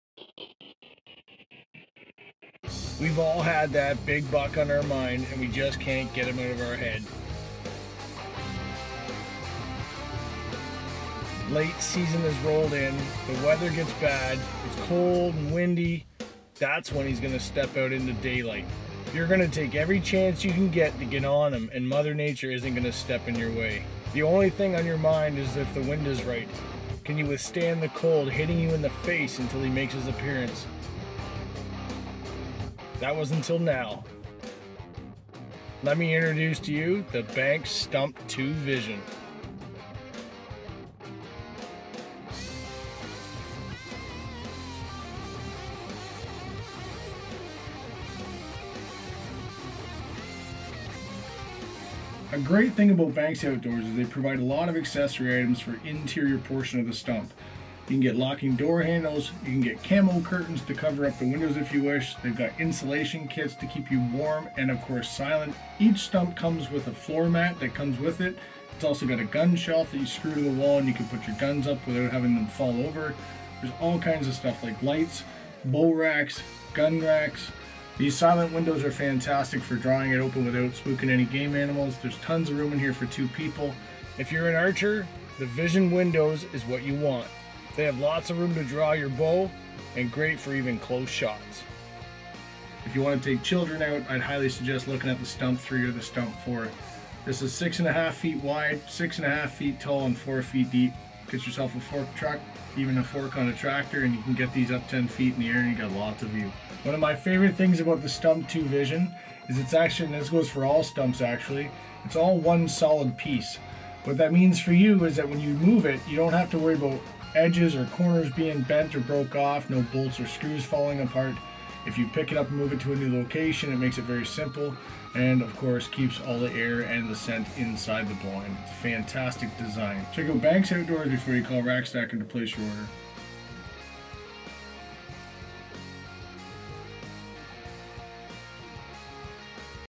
Elevated hunting blind review in Canada
Steve Elmy gets out in the cold and shows you in full detail why you should consider getting into a Banks Stump blind for hunting this coming season